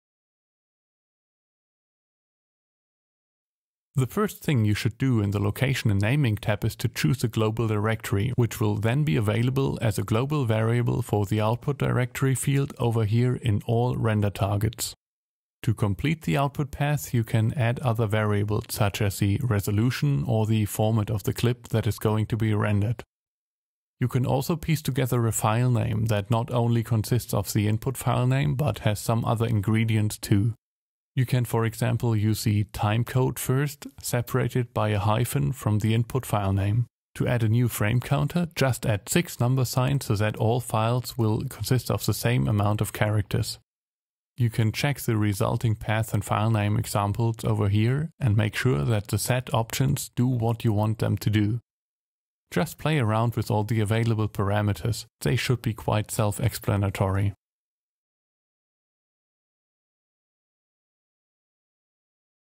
ARRI Tech Talk: ARRIRAW Converter - Location and Naming
Explains the configuration options that you have to set up the path and file name in a render target.